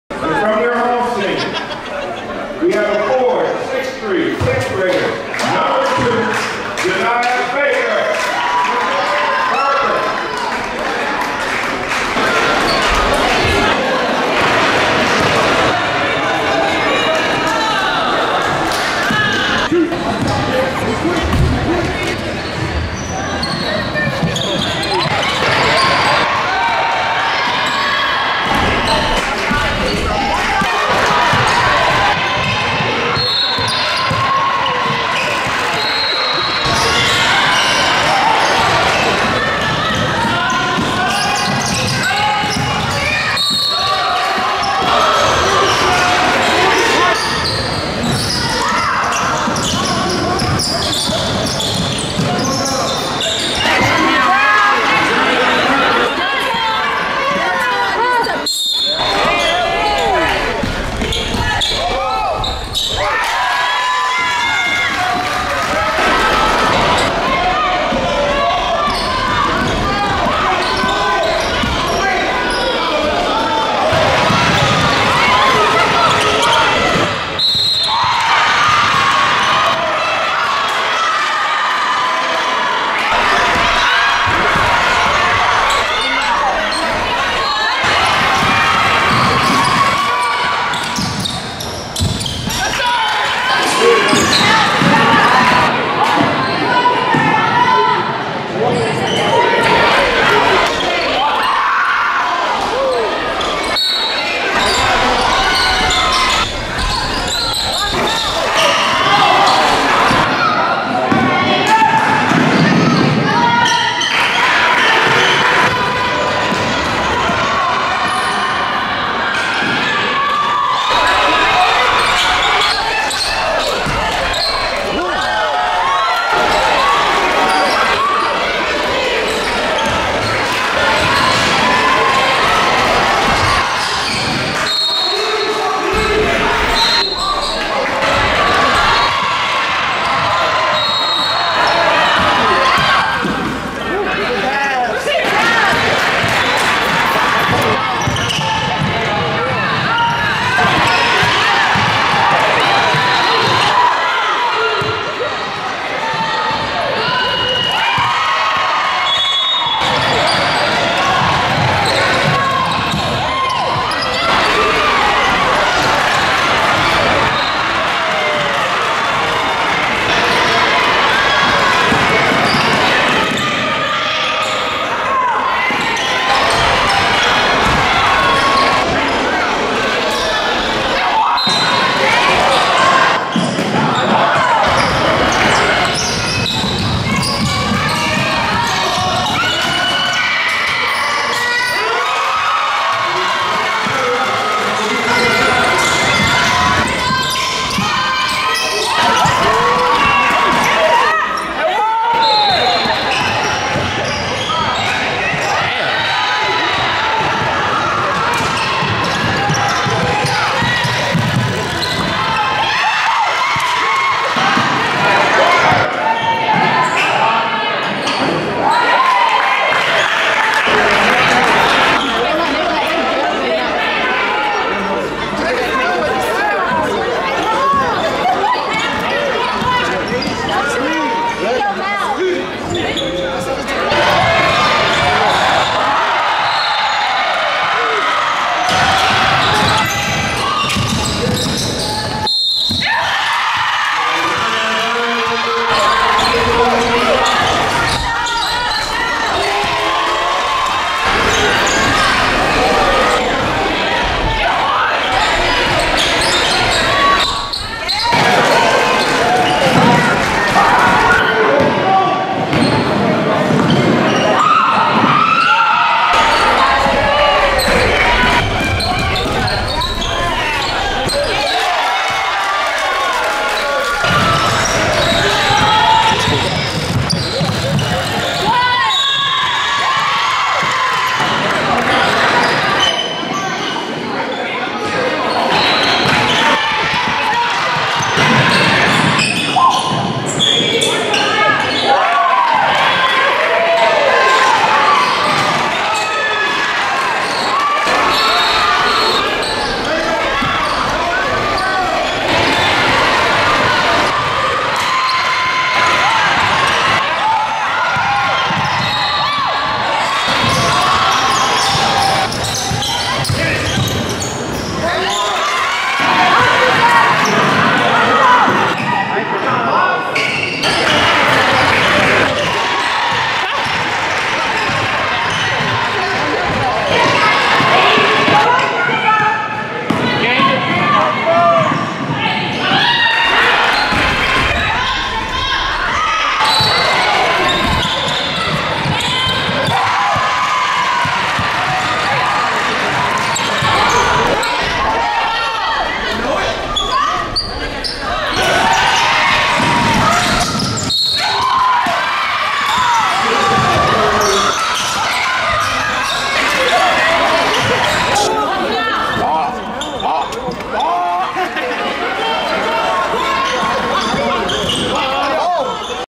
Janiah Barker is THE #1 PLAYER IN HER CLASS! FULL SOPHOMORE SEASON HIGHLIGHTS!
SOPHOMORE YEAR STATS:
11.9 PPG
7.5 RPG
1.9 BPG
1.5 SPG
1.9 APG